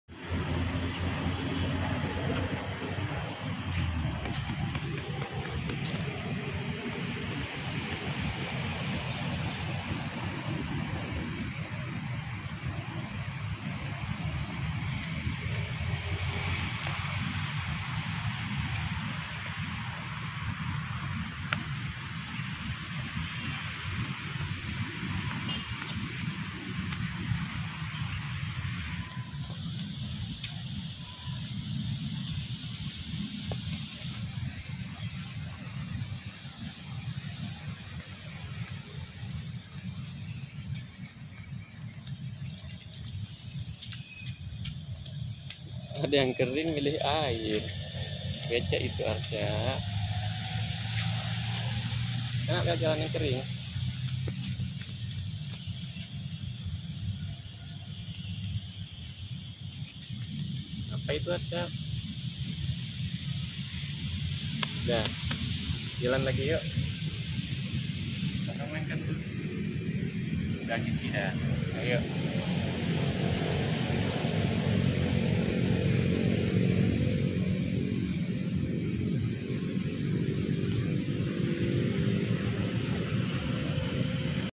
petualangan arsya 3
jalan jalan sambil olahraga berpetualang mengenal alam